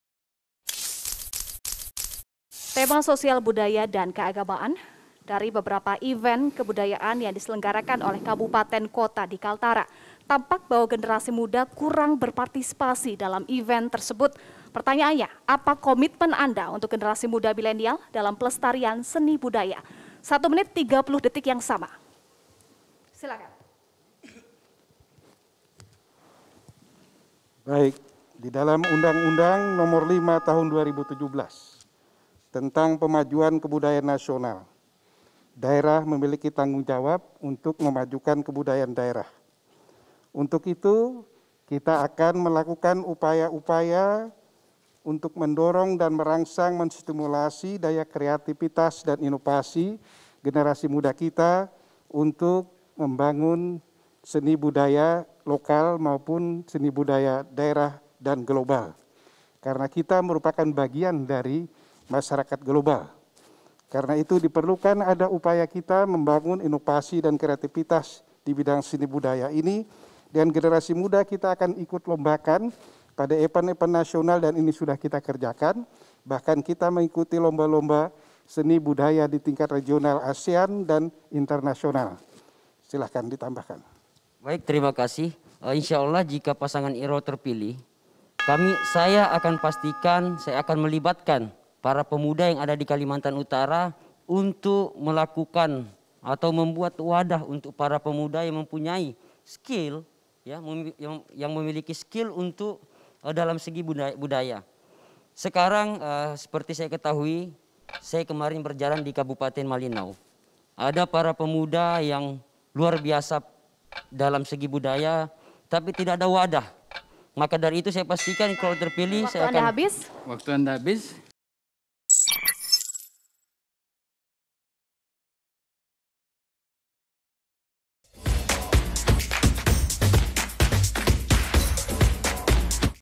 Dorong Pelestarian Budaya, Irianto Irwan Ingin Libatkan Generasi Muda
Calon Gubernur Kalimantan Utara nomor urut 2, Irianto Lambrie-Irwan Sabri mengajak generasi muda untuk aktif dalam pelestarian budaya di Kalimantan Utara. Irianto mengatakan pihaknya akan menjaring lebih banyak pemuda untuk aktif dalam berbagai kegiatan seni budaya sekaligus memupukan rasa cinta tanah kelahiran.

Irianto menjelaskan diperlukan minat bersama untuk membuat inovasi dan kreativitas di bidang seni budaya. Generasi muda menjadi salah satu elemen penting yang akan dilibatkan dalam misi Irianto tersebut.

Sementara wakilnya, Irwan Sabri, mengatakan pihaknya akan merangkul para generasi muda dengan berbagai kegiatan seni daerah. Dia juga akan menampung para pemuda yang mempunyai minat lebih dalam menjalankan berbagai budaya dan kesenian yang ada di Kaltara.

"Saya akan pastikan melibatkan para pemuda yang ada di Kalimantan Utara untuk membuat wadah untuk para pemuda yang mempunyai skill dalam segi budaya. Saya kemarin berjalan di Kabupaten Malinau, ada para pemuda yang luar biasa dalam segi budaya tapi tidak ada wadah, maka dari itu saya pastikan kalau terpilih saya akan mewadahi hal seperti itu," ungkap Irwan.